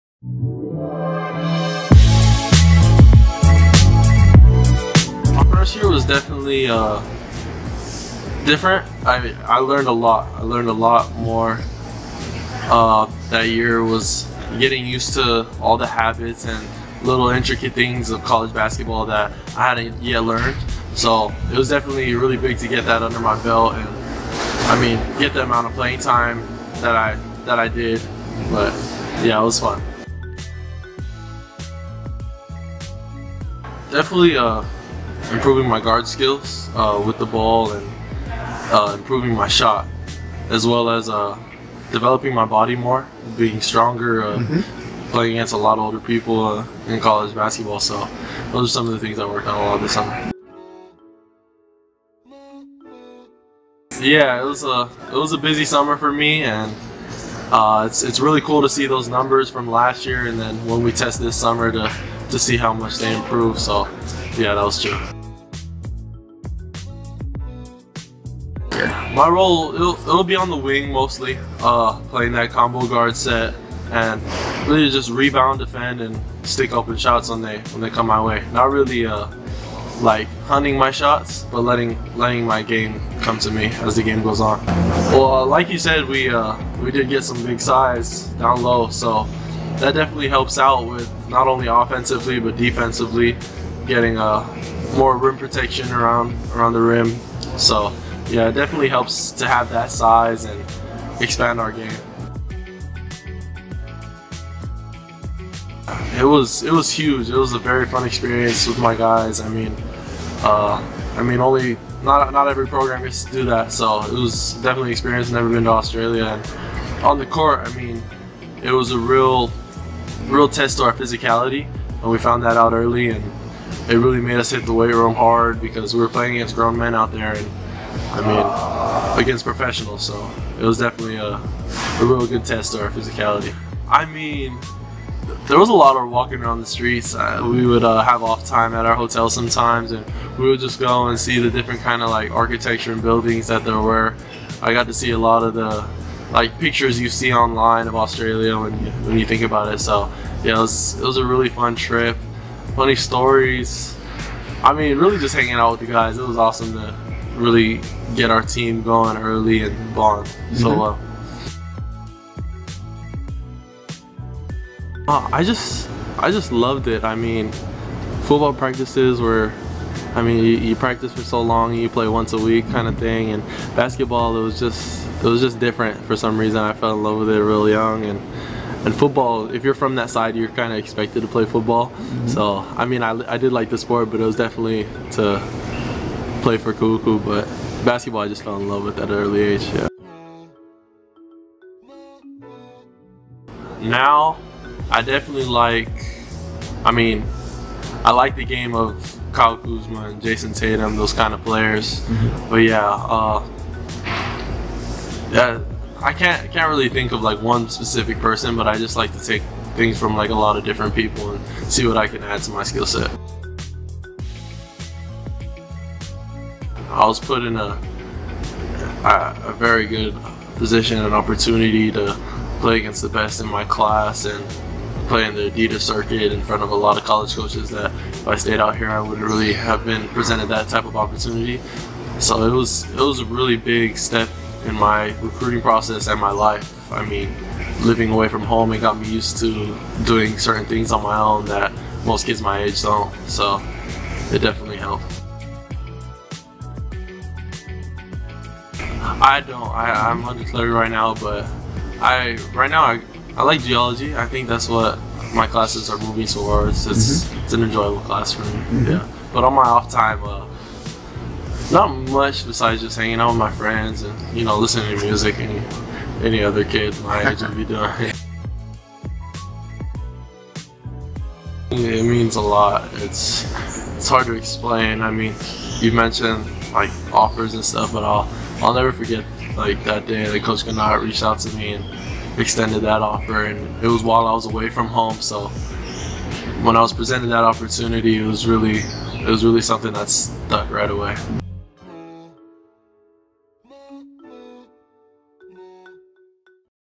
SAMUTA AVEA
UH basketball super soph Samuta Avea looks to have a breakout season for the Rainbow Warriors. In this exclusive ESPN 1420 interview, the Kahuku High School alum talks about his expectations for the 2018-19 season; wearing the green-and-white; choosing hoops over football; and more.